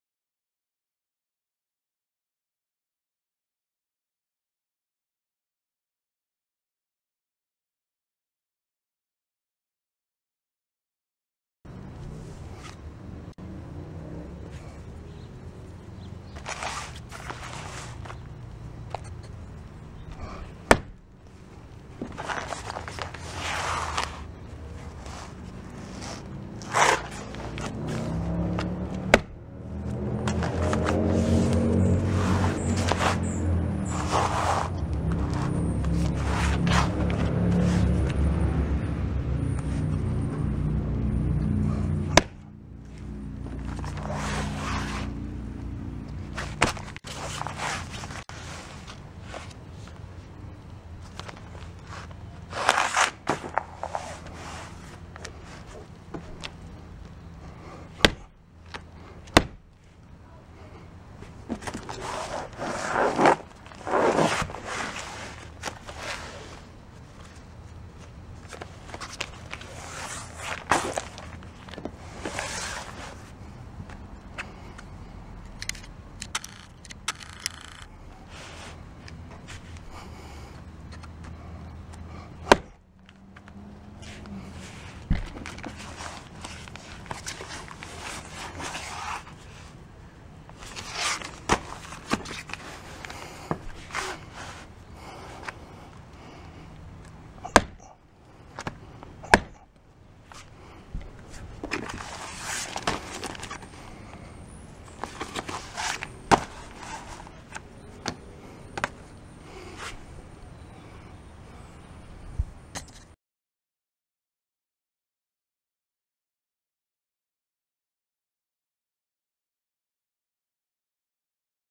Skarr Armor IIIA Kevlar Vest vs. Ice Pick
Skarr Armor SKVC-01 certified Kevlar IIIA vest vs a $9 Walmart ice pick.  This vest is not rated stabproof.

Ice Pick